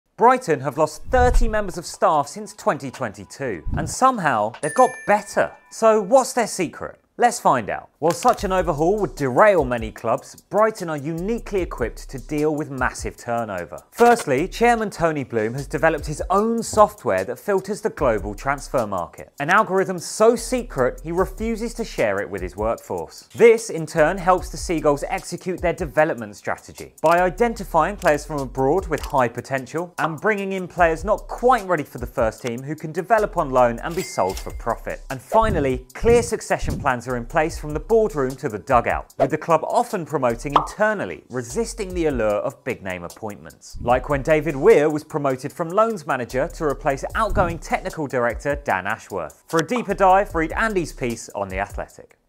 How Brighton Lost 20 Players, 10 Staff, And Got BETTER 📈
Welcome to The Athletic FC, where quality football journalism meets bitesize video.

Brighton are a famously well run club, demonstrated by their ability to improve after losing players and managers. But how do they do it?